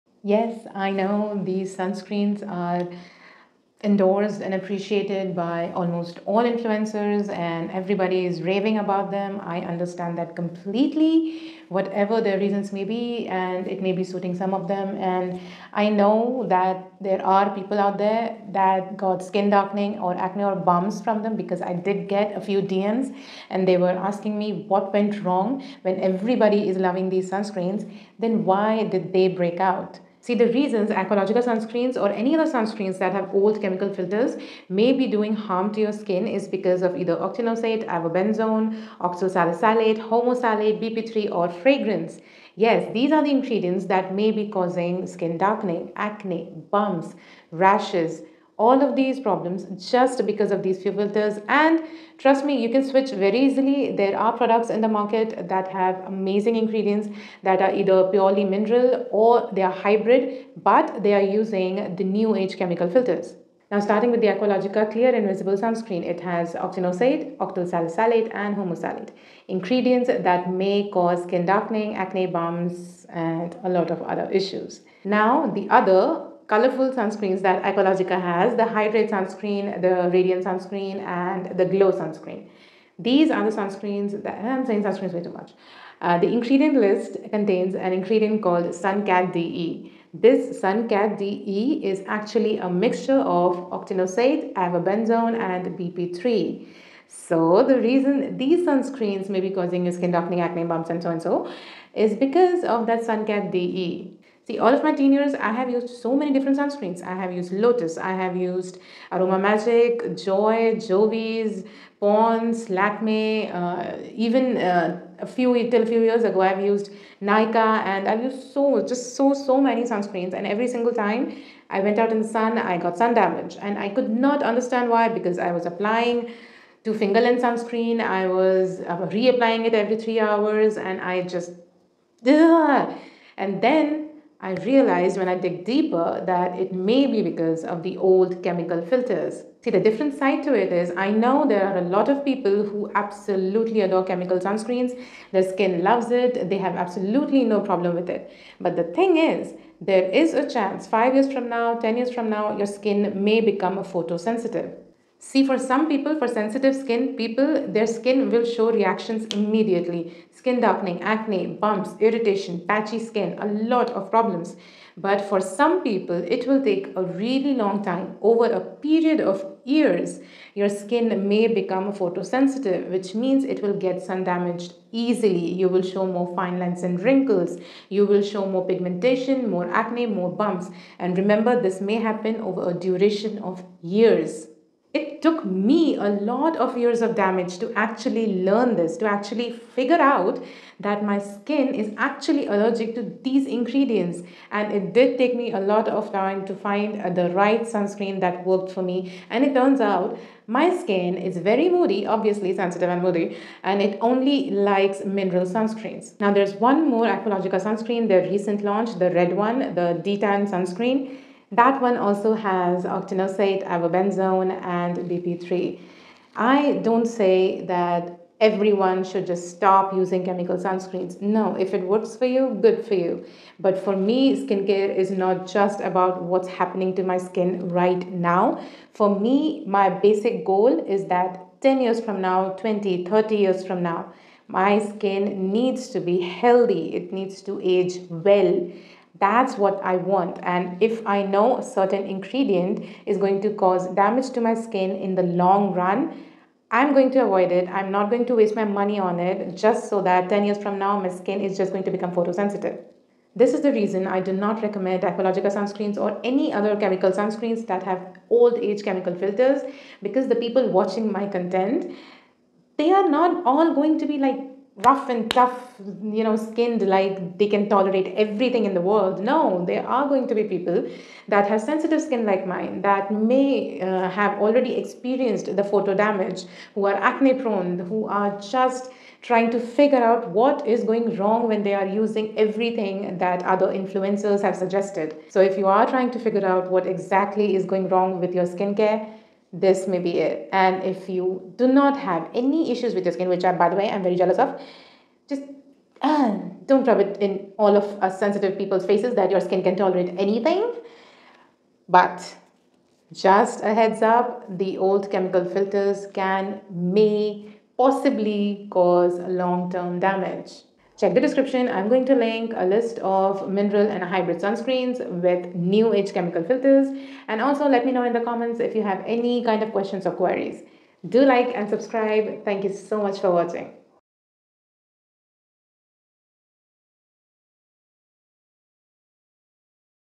Why I Stopped Purchasing These SUNSCREENS!!
Earth Rhythm Ultra Defence Hybrid Sunscreen :

Vilvah Melt in Milk Sunscreen :

Skinvest Sunny Side Up Sunscreen :

Derma Essentia Mineral Sunscreen Gel :

Vaunt Hybrid Sun Defence :

Put Simple Beat the Sun Sunscreen :

Re'equil Sheer Zinc Sunscreen :